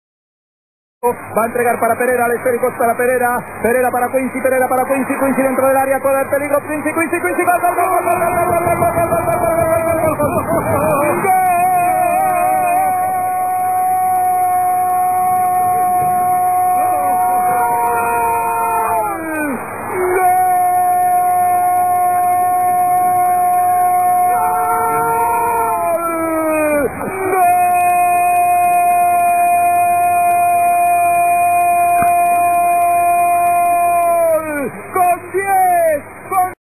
Gol Quincy R.C. Celta - Sevilla Atlético ( 2 - 1)
Gol de Quincy Owusu, xogador do Celta de Vigo no partido fronte ao Sevilla Atlético correspondente á 11ª xornada de liga da temporada 2007/08.